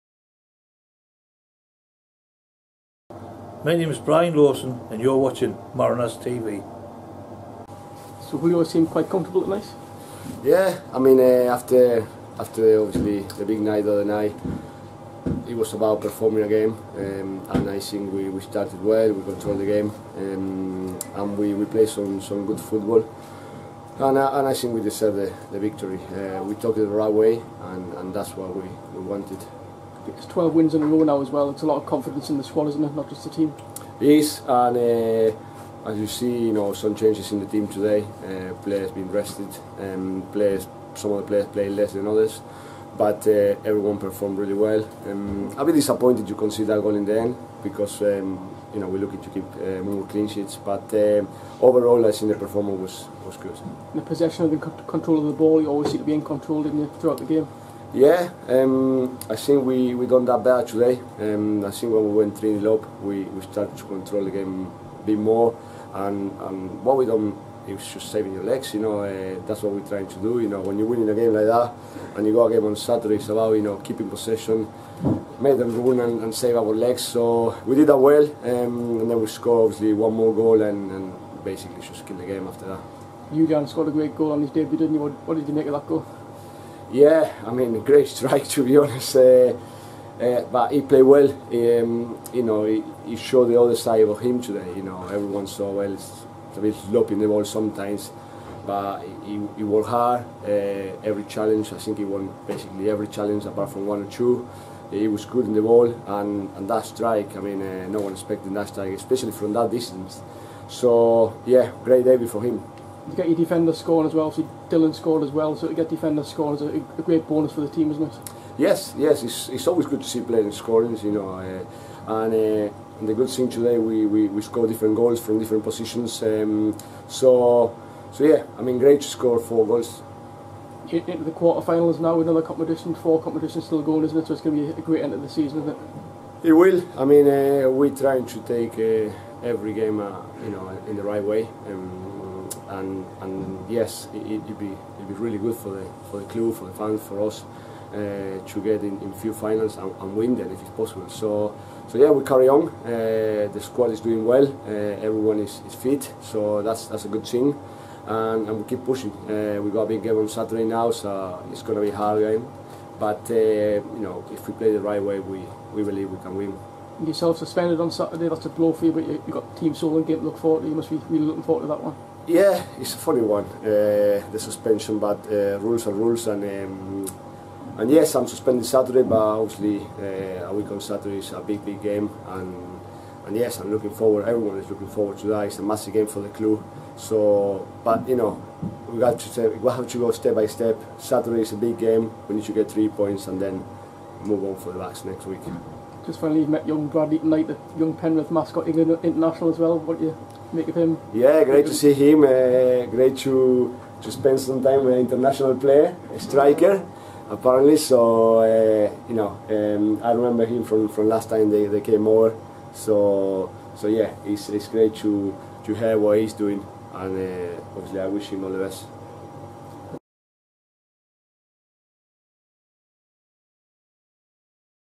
Julio Arca reflects on South Shields' League Cup win over Penrith
Julio Arca gives his reaction to South Shields' 4-1 win over Penrith in the League Cup at Mariners Park.